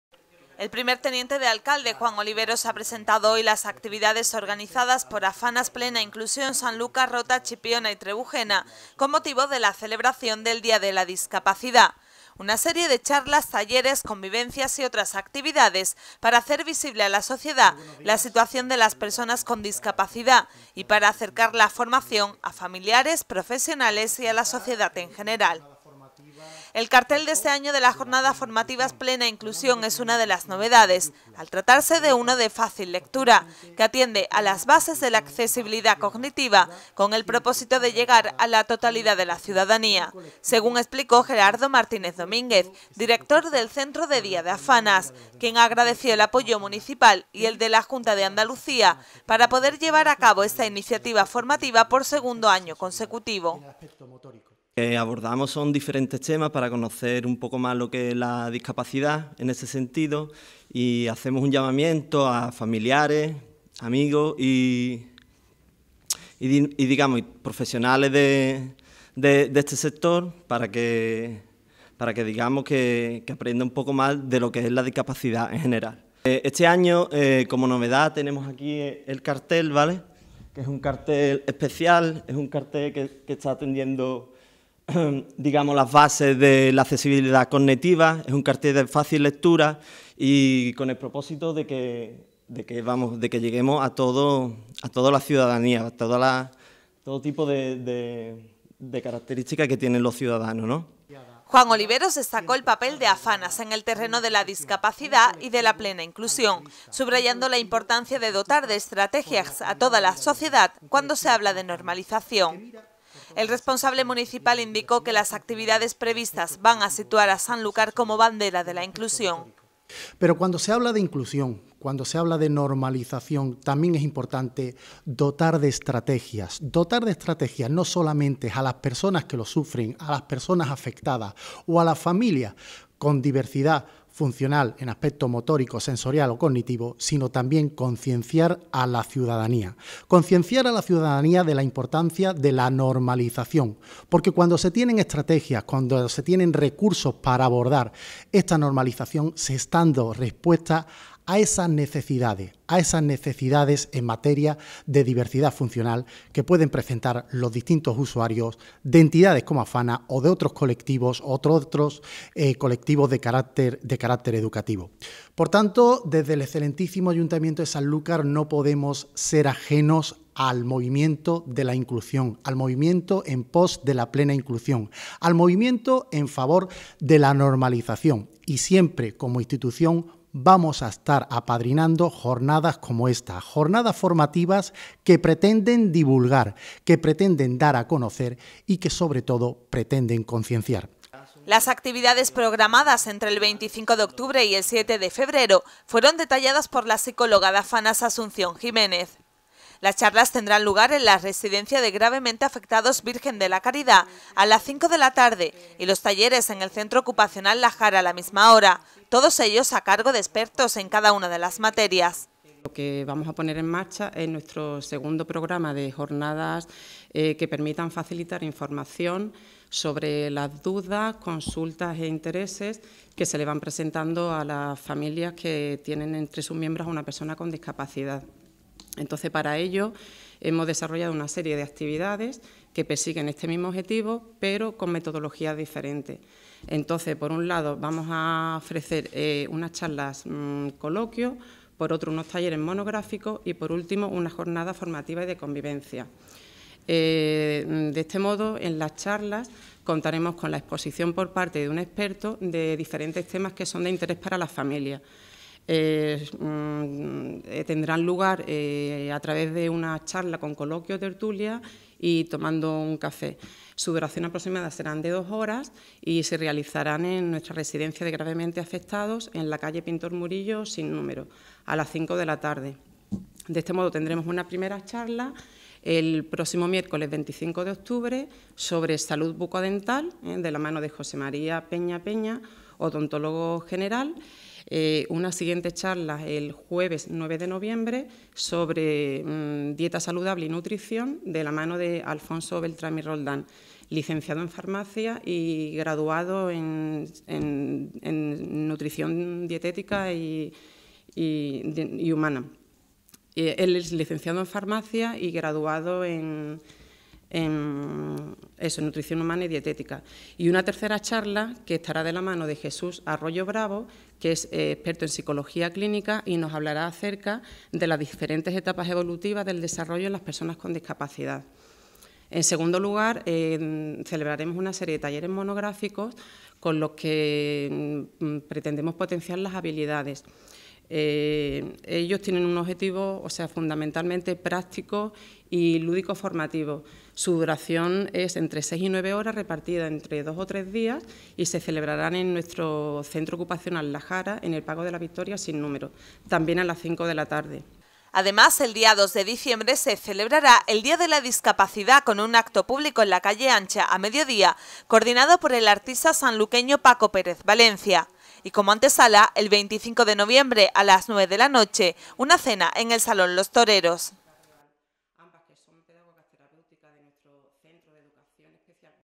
Noticias, Charlas y talleres en las II Jornadas Formativas Plena Inclusión Afanas
El próximo 3 de diciembre se celebrará de nuevo el Día de la Discapacidad, enmarcado en esta celebración llega un amplio programa con las II Jornadas Formativas Plena Inclusión de Afanas.